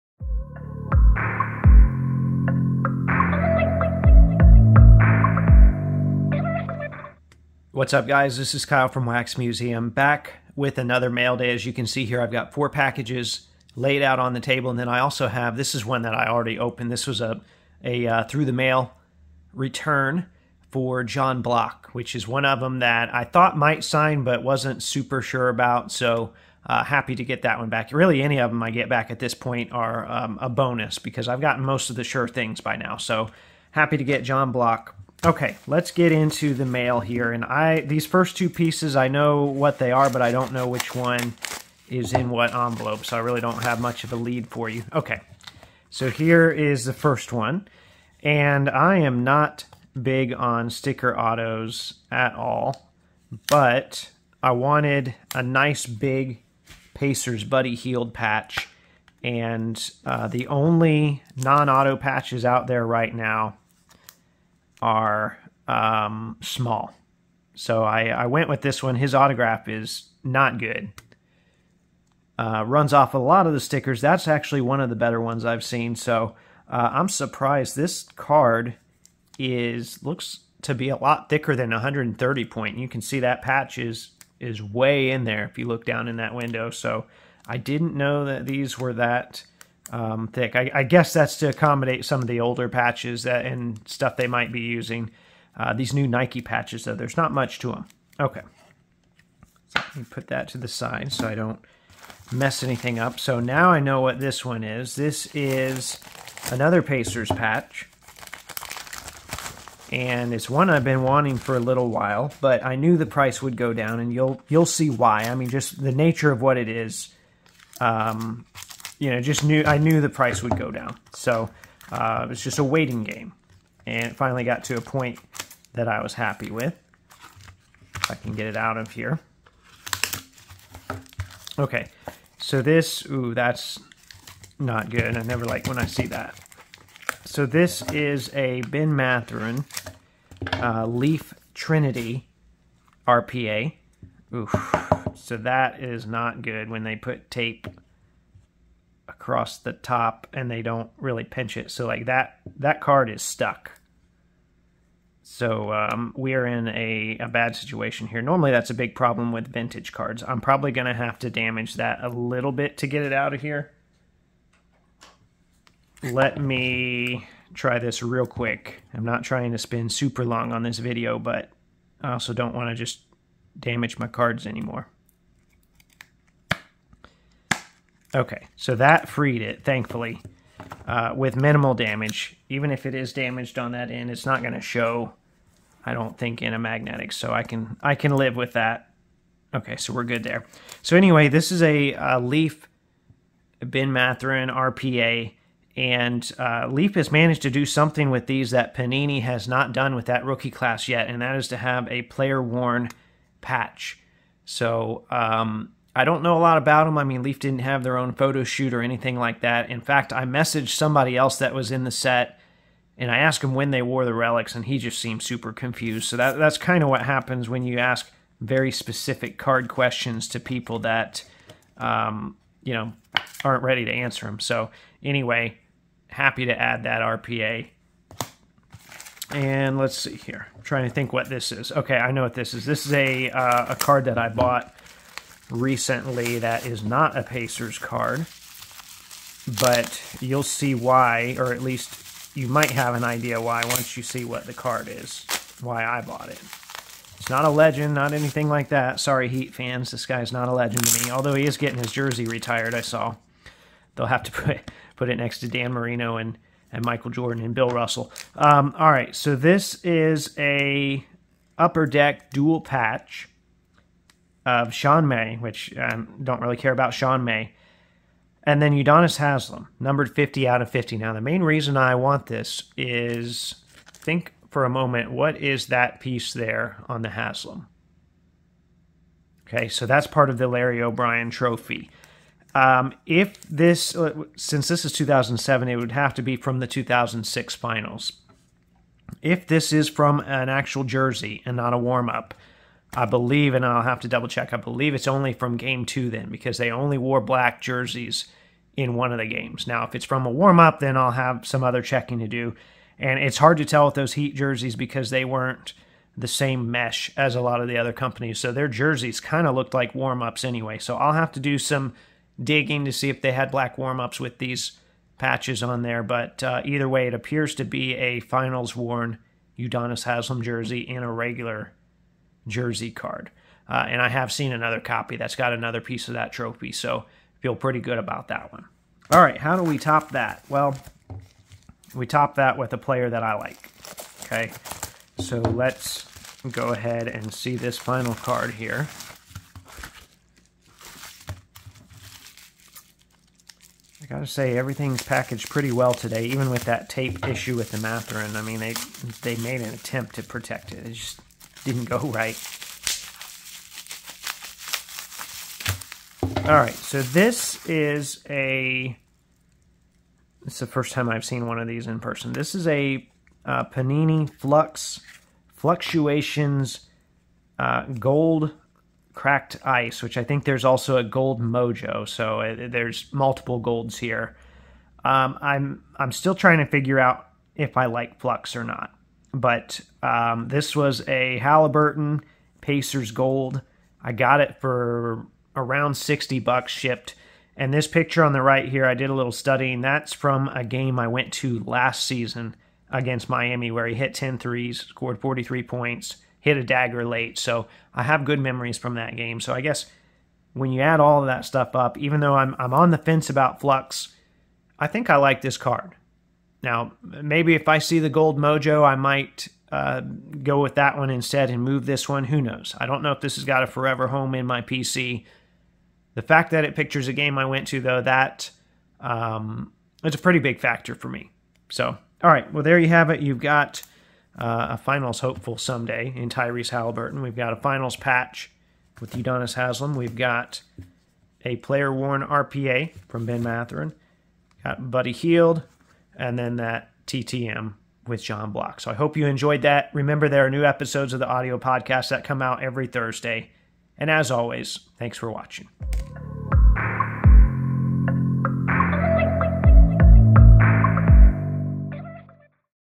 Catching Up On Mail (Haliburton Gold) ✉️ I Almost Lost This Mathurin RPA to the Packing Tape Gods...
This was a busy mail week! A few new items, but more of the same kind of stuff!

Check out new episodes of the Wax Museum Podcast every Thursday